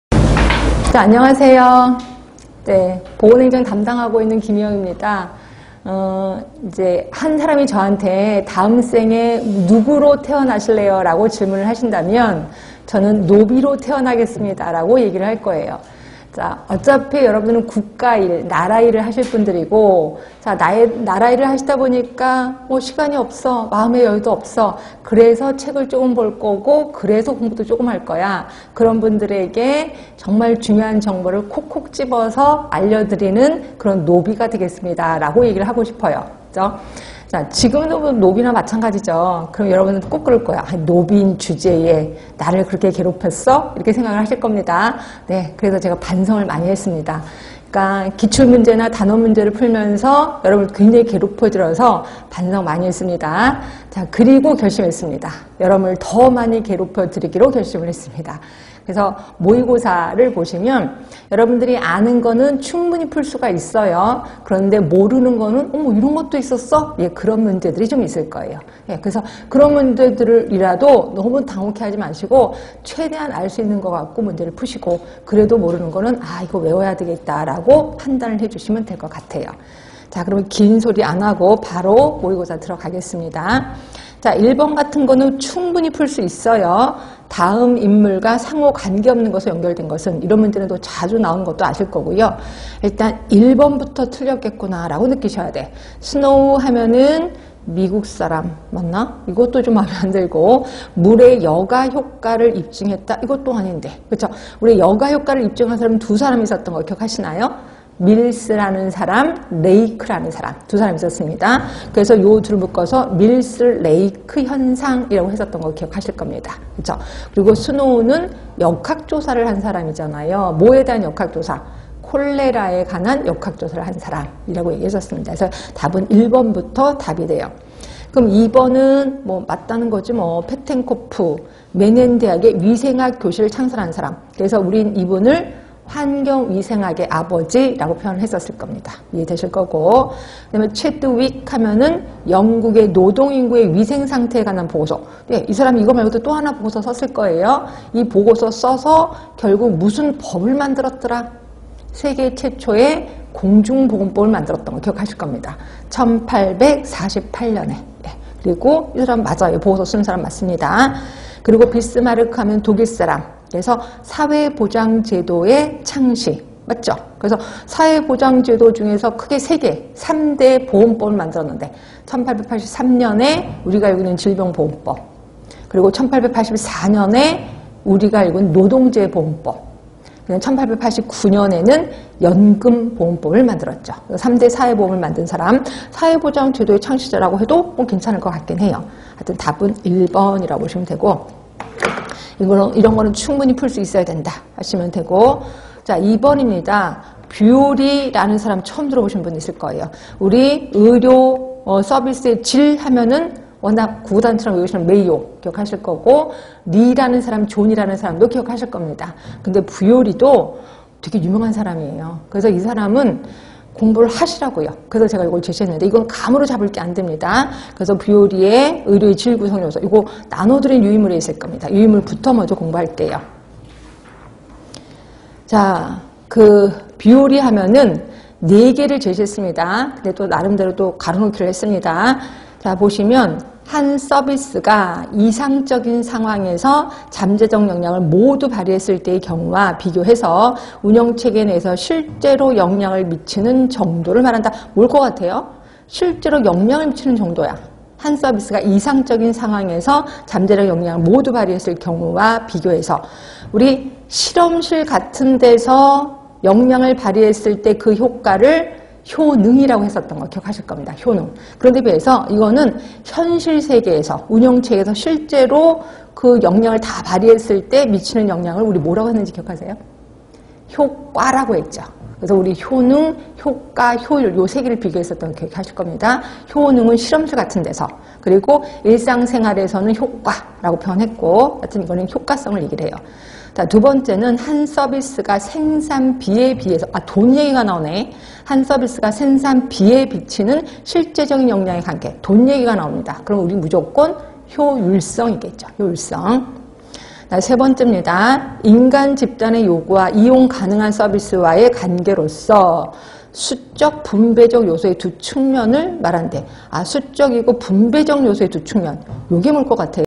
[전공은 대방열림] 9급 보건직 보건행정 실전모의고사 2022년 시험대비 문제풀이 강의
2022년 보건직공무원 시험대비 김희영 교수의 공중보건 보건행정 실전모의고사 문제풀이 강의